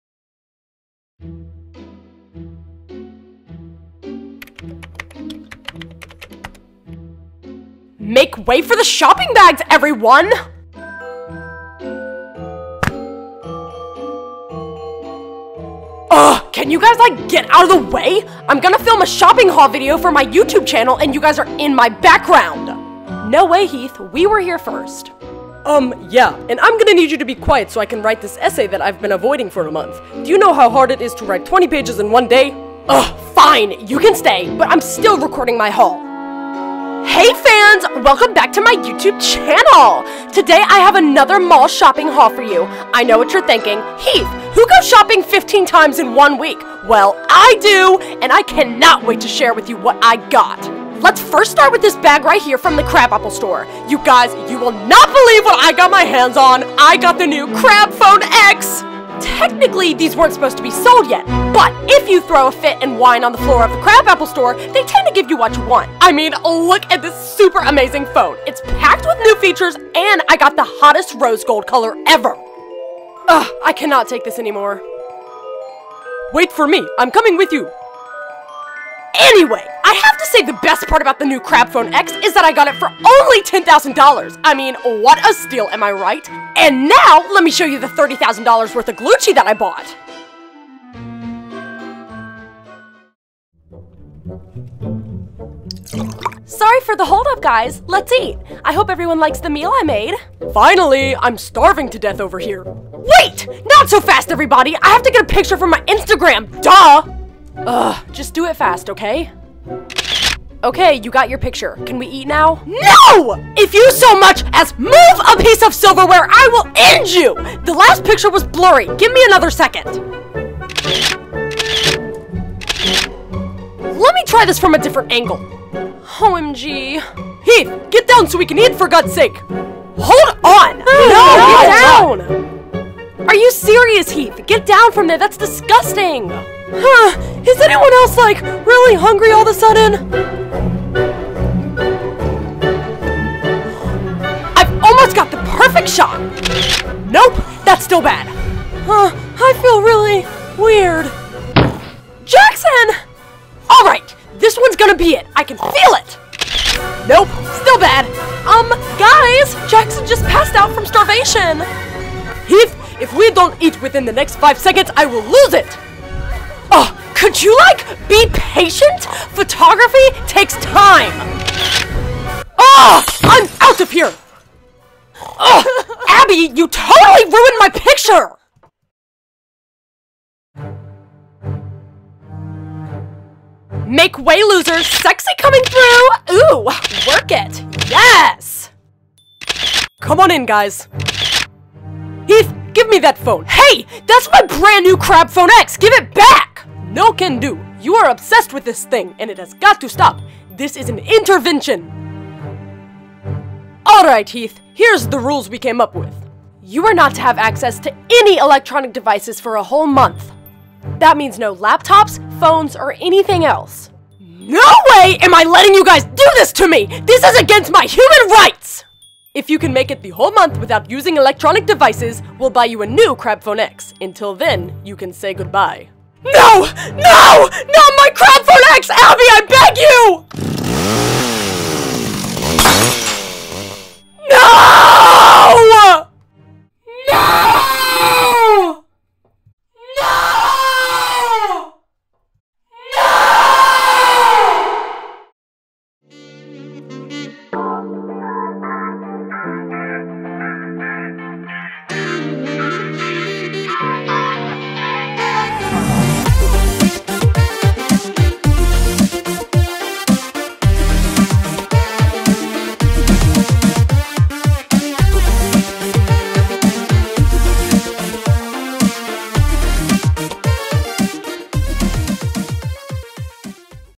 Heath's Cellphone Addiction- A MH/EAH stop motion
Heath gets a new cellphone and the ghouls have to intervene when he becomes addicted.

FAQ:
Q:  What program do you edit with?
A: IMovie on my computer!

Ask me more questions in the comments and I will answer them here!